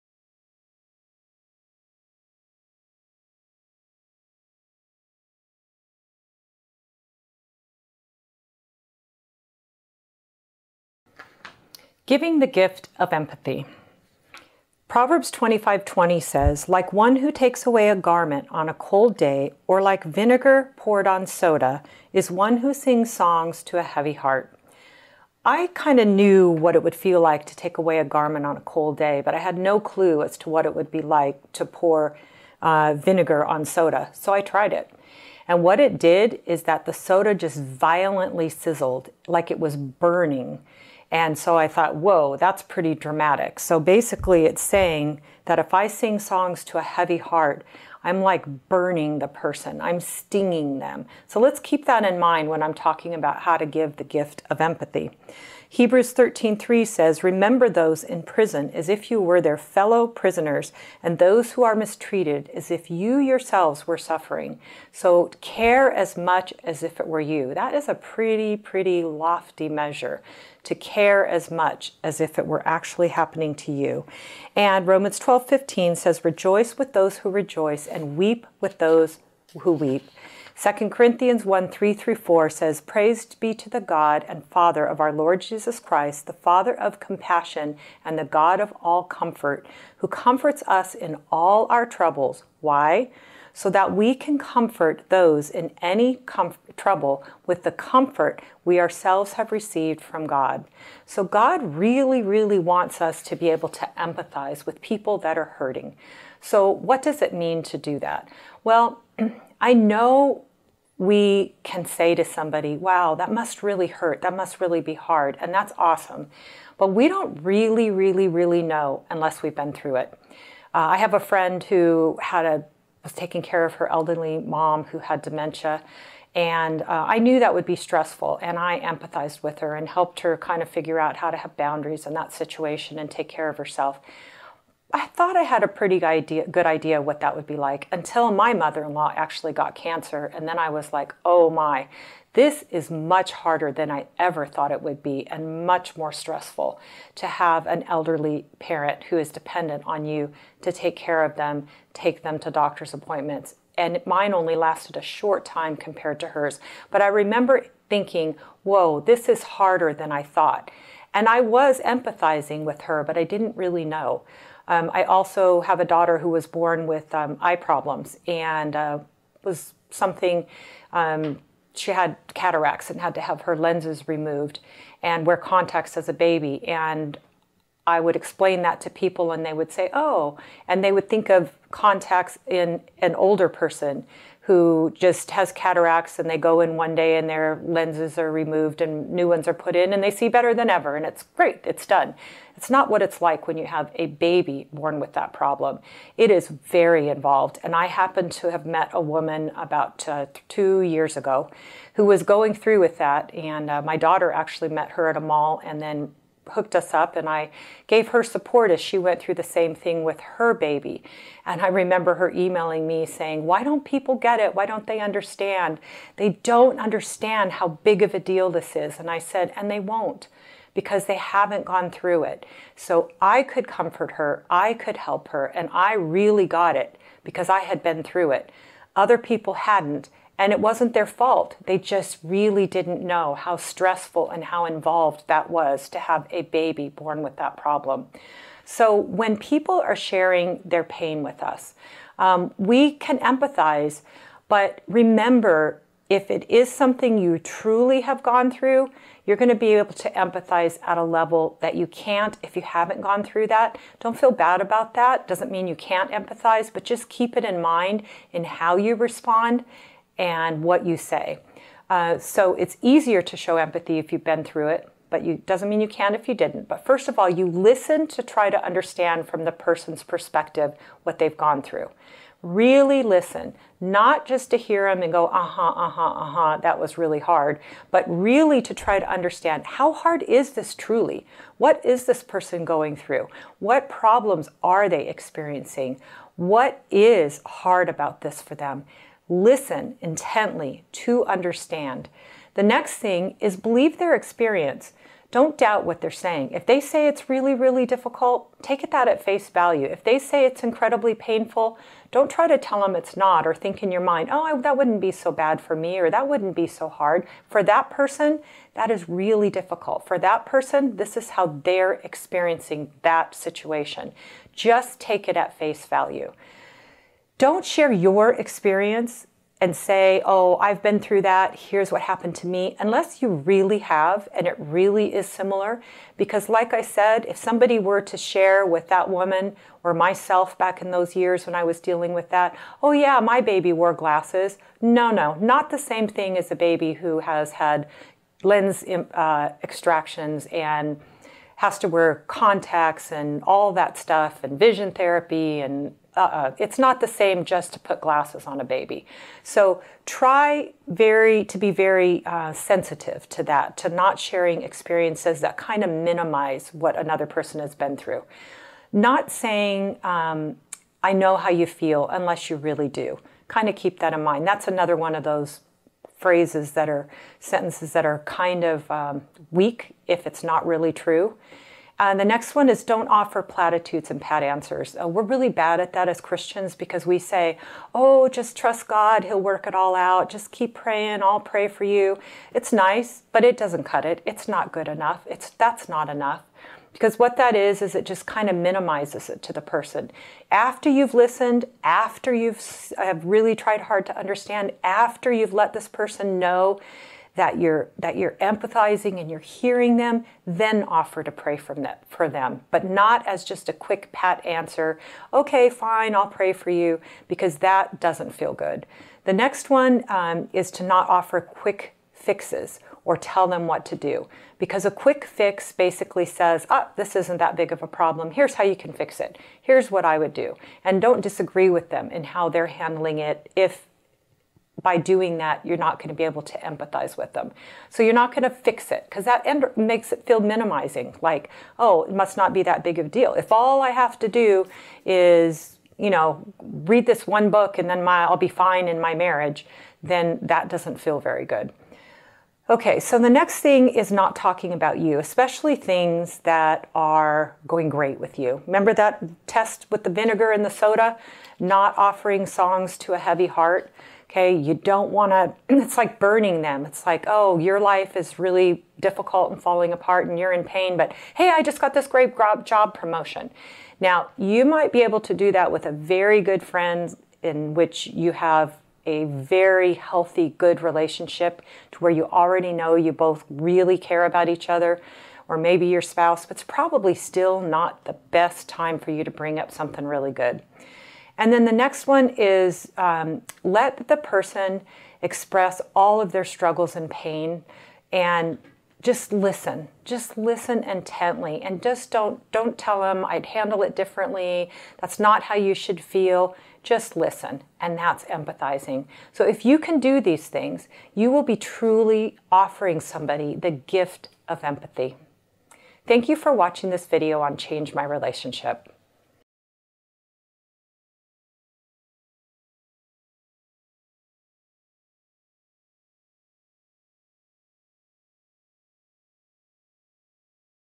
Giving the Gift of Empathy
When people are hurting, giving the gift of empathy is what you want to strive for; yet, there are common mistakes we make. Learn how you can give the gift of empathy in these simple ways...